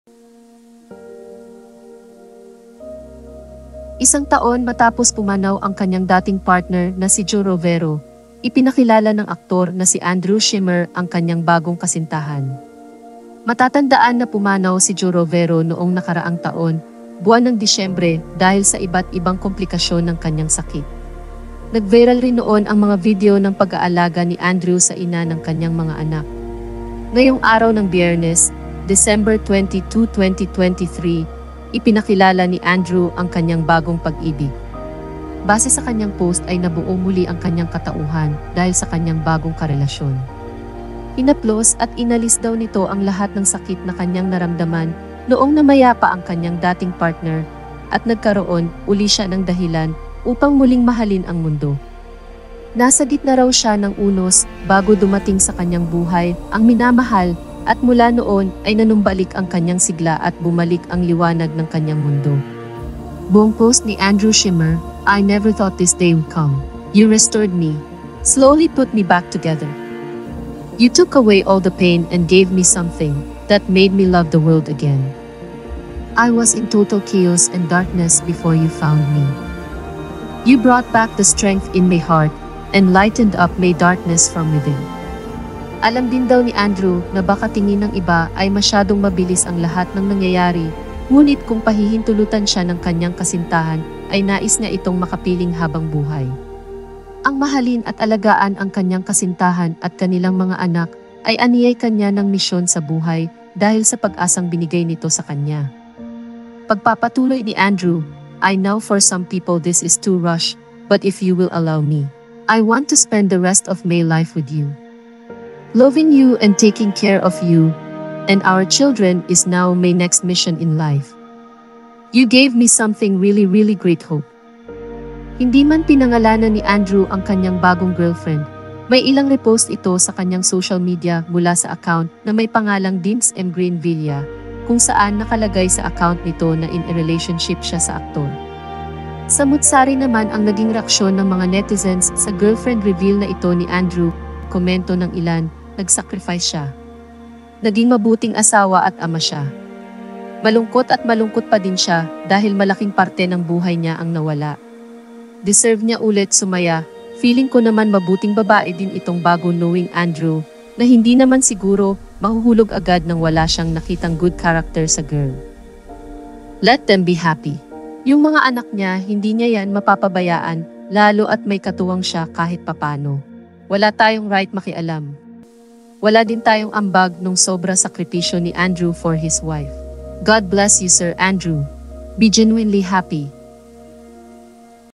Andrew Schimmer, pinasalamatan ang bagong girlfriend sa pag-asang binigay nito sa kanya!
Andrew Schimmer, pinasalamatan ang bagong girlfriend sa pag-asang binigay nito matapos pumanaw ang dating partner!